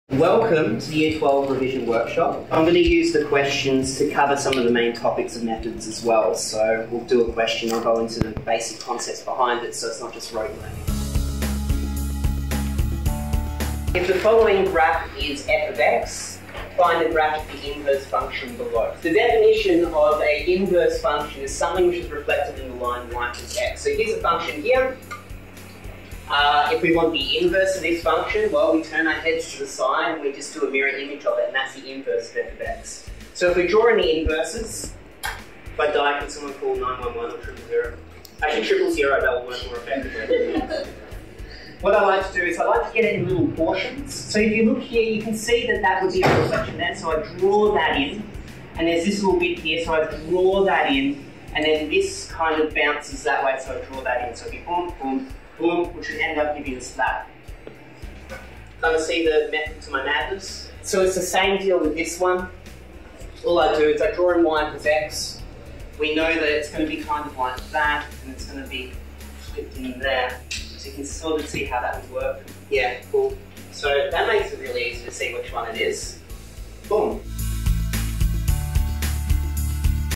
Maths Methods Workshop (Sample) - Inverse Functions
Come to the next workshop!

This is just a short multiple choice question, I also cover extended response questions as well and all the concepts you need to know to excel in VCE Maths Methods.